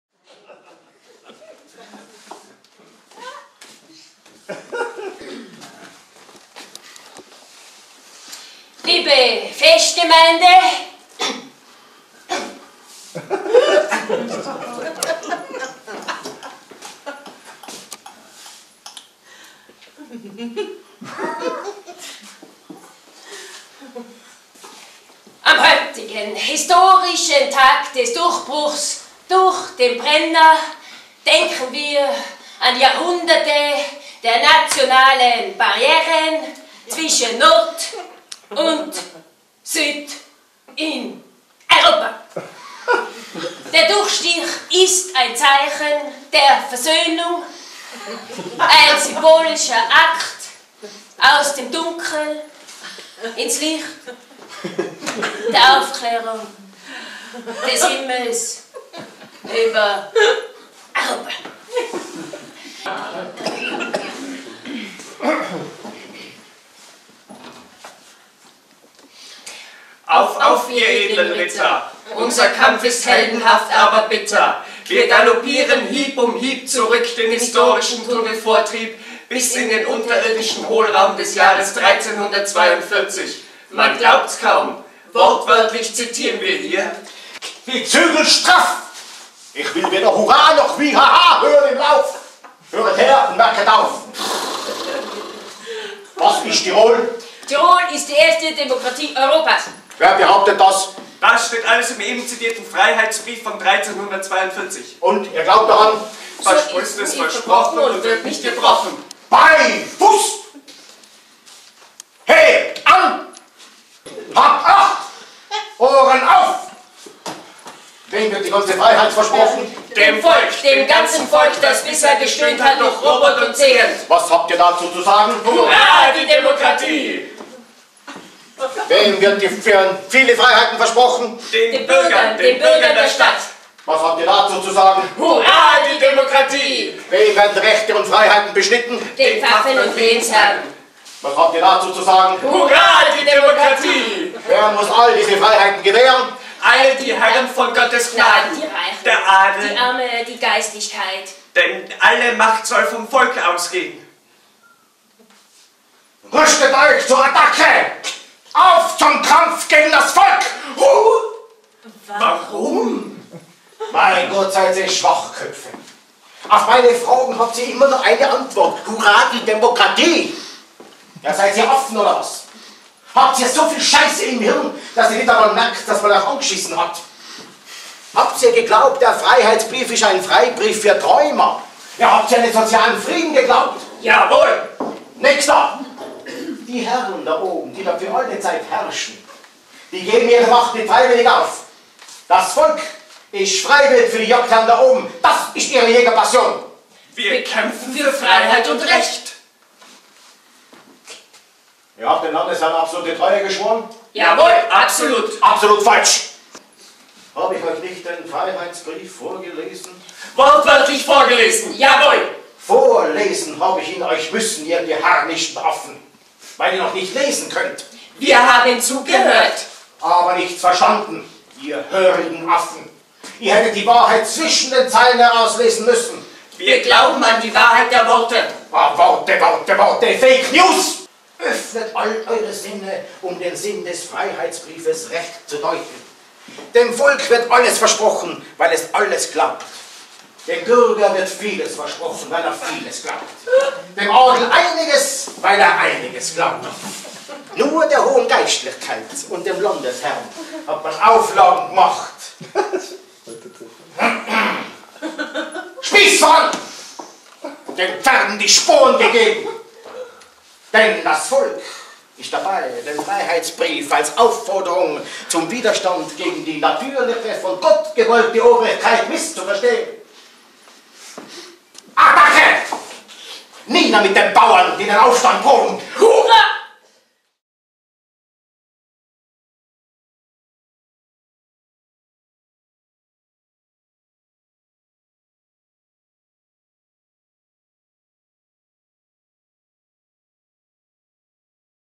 Schluss mit dem Hofertheater
Szenenausschnitt (Probenstadium) aus "Schluss mit dem Hofertheater" - aufgenommen bei den "StubenSpielWelten" 2019 in
Kauns